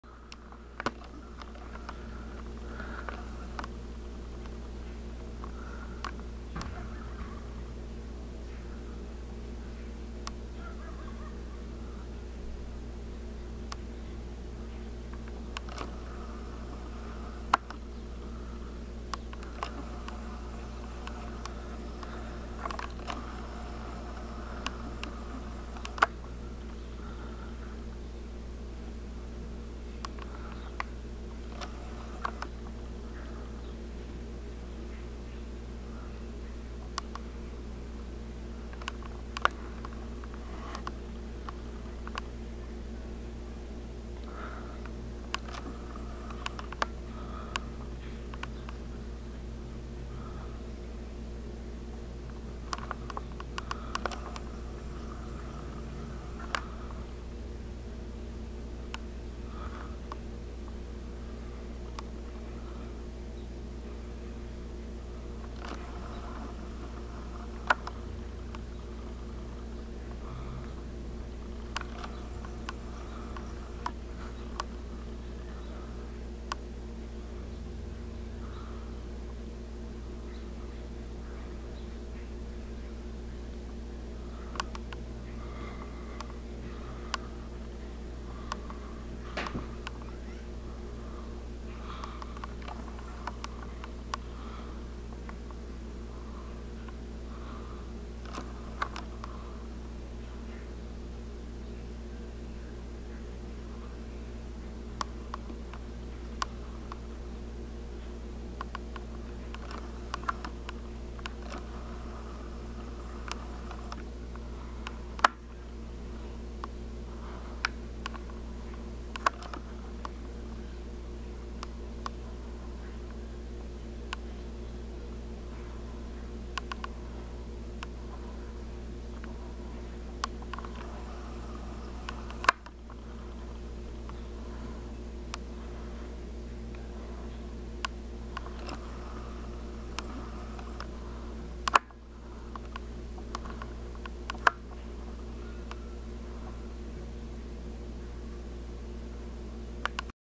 Magpie (bird) mocks Papillon (dog)
Magpie teasses Papillon in back yard. In the end, Magpie 1, Papillon 0. This occurred in our back yard in Helena, MT. on 3/31/14.